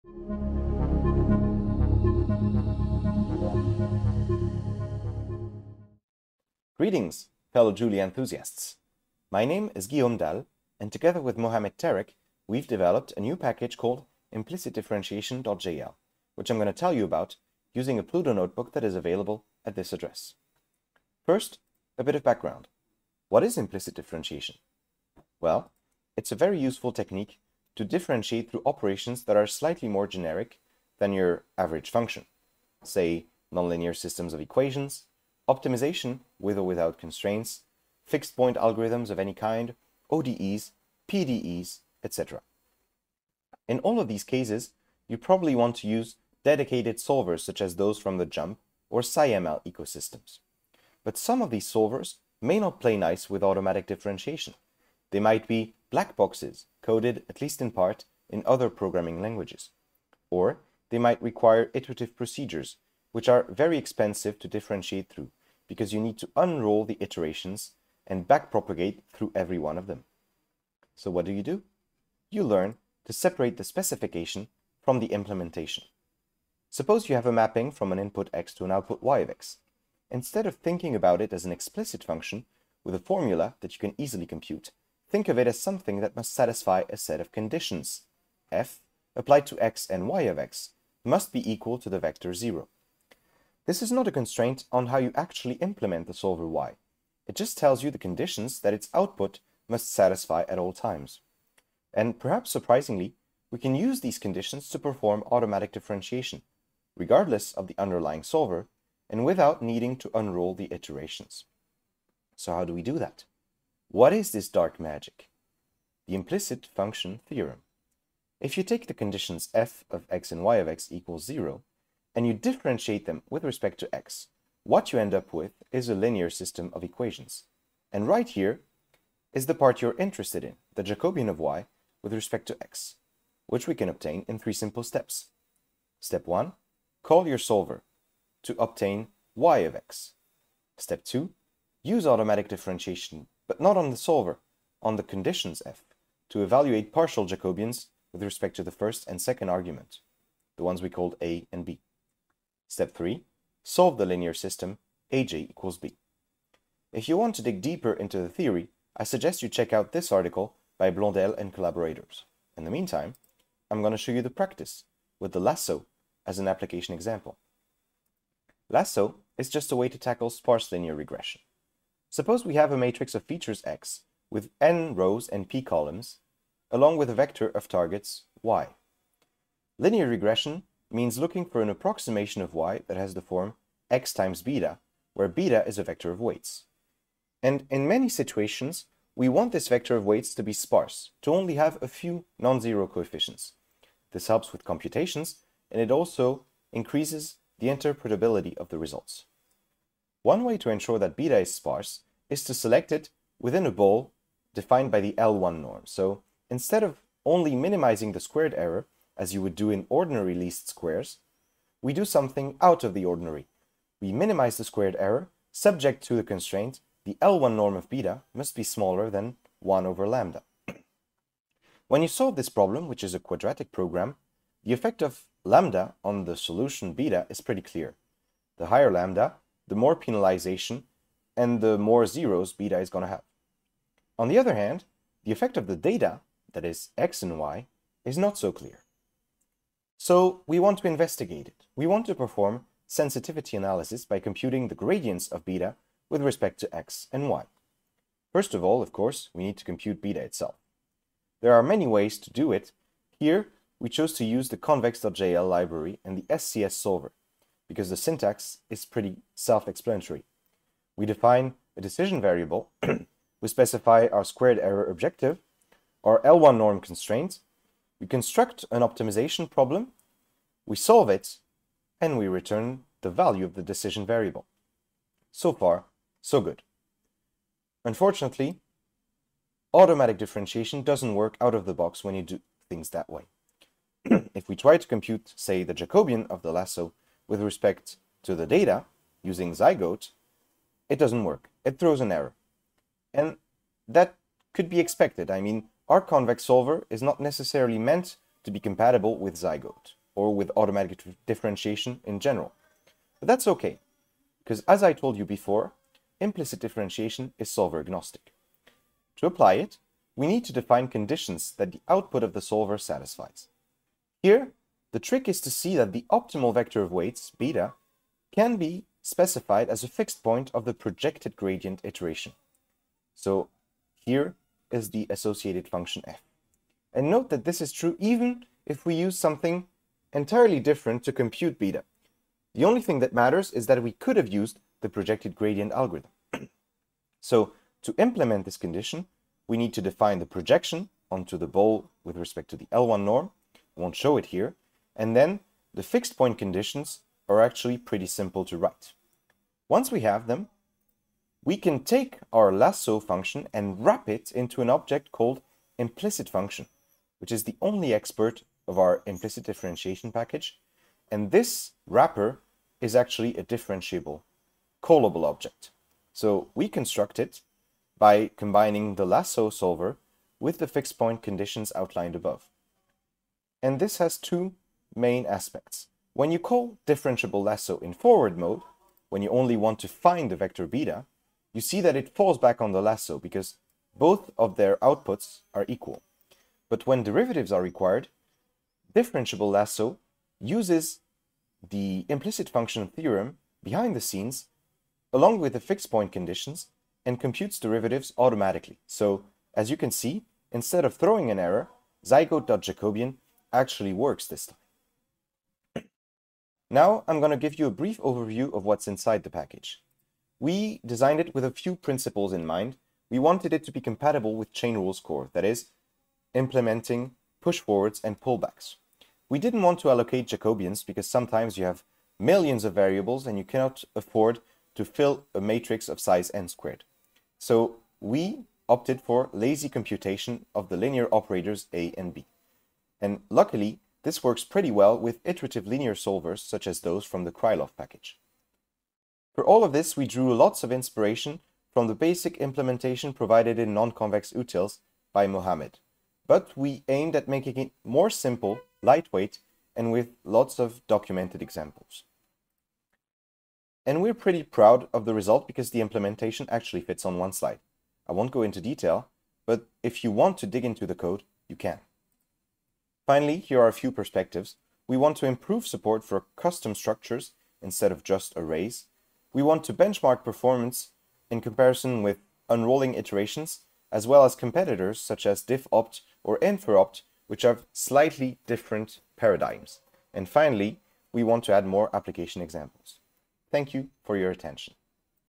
ImplicitDifferentiation.jl: Differentiating Implicit Functions | M. Tarek, G. Dalle | JuliaCon 2022
We present a Julia package for differentiating through functions that are defined implicitly. It can be used to compute derivatives for a wide array of "black box" procedures, from optimization algorithms to fixed point iterations or systems of nonlinear equations. Since it mostly relies on defining custom chain rules, our code is lightweight and integrates nicely with Julia's automatic differentiation and machine learning ecosystem.

00:00 Welcome!
00:10 Help us add time stamps or captions to this video! See the description for details.